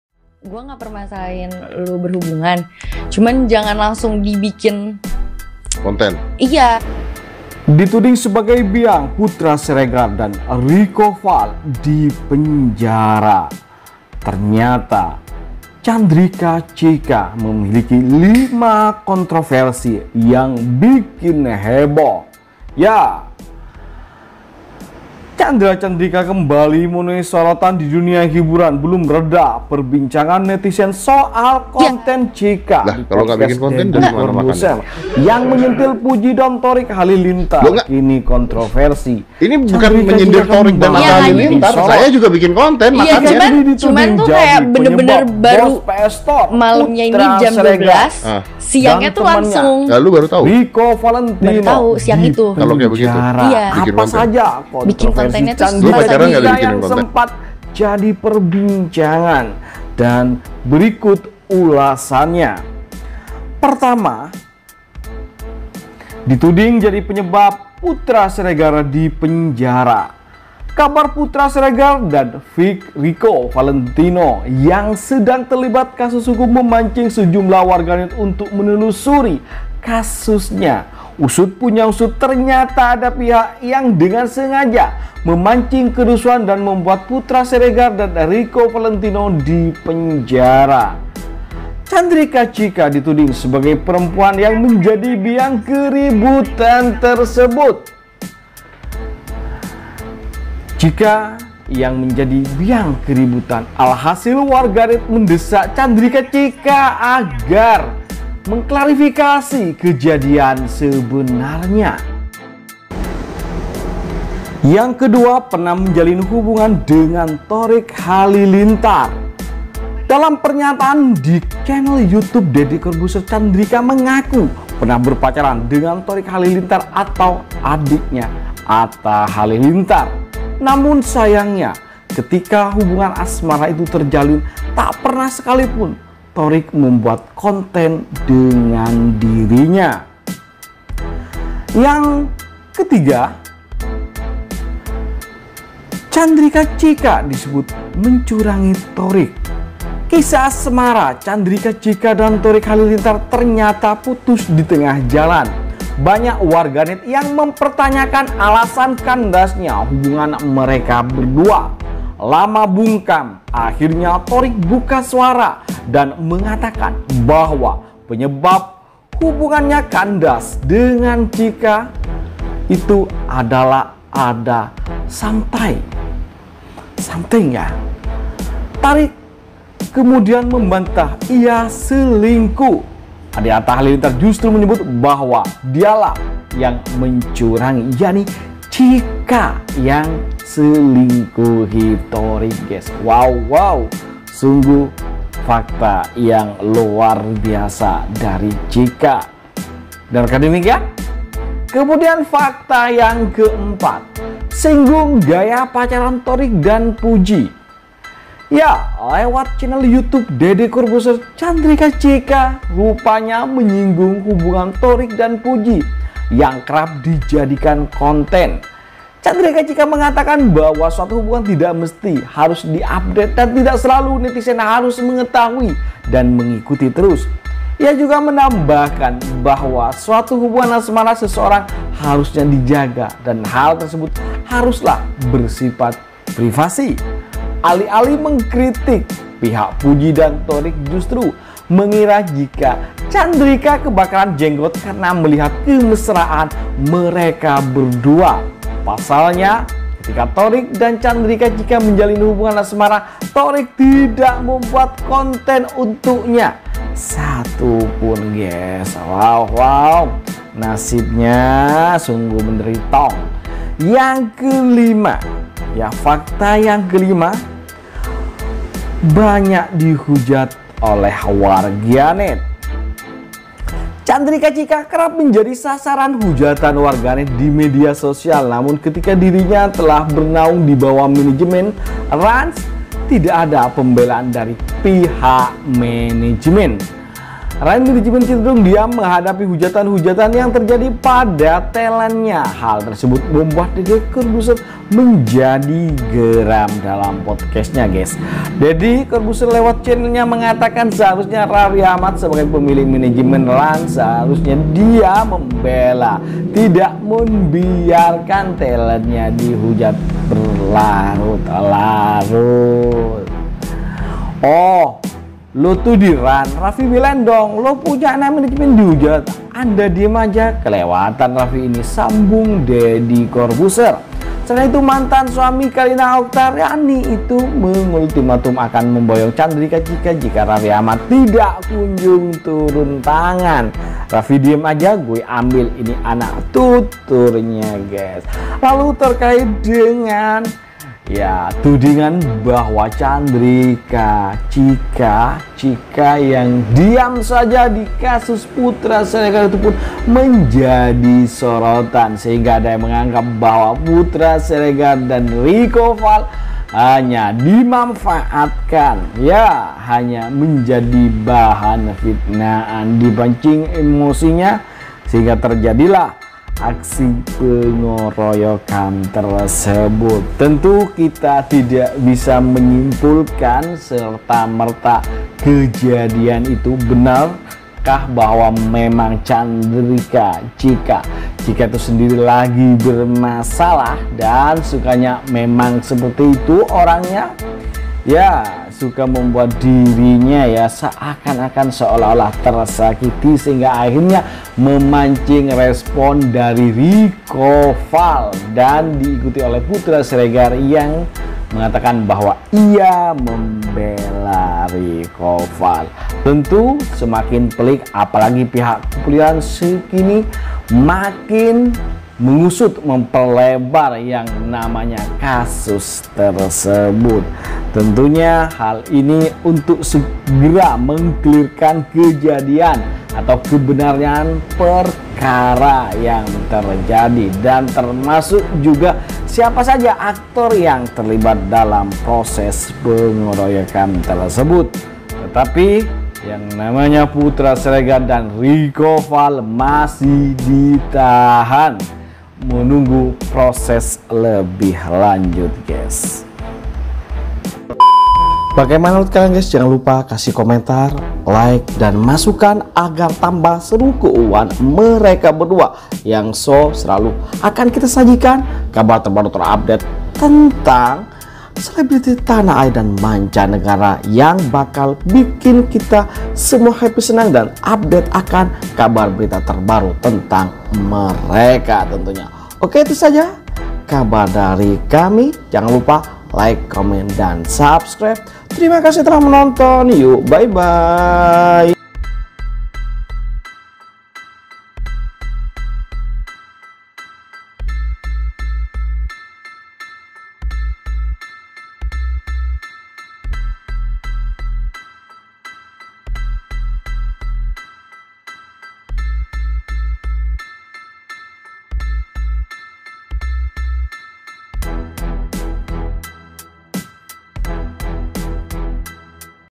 Dituding Biang Putra Siregar DiPENJARA❓️ Ini 5 Kontroversi Chandrika Chika yg Bikin HEBOH ❓️
Jangan Lupa: _
Thankz!!..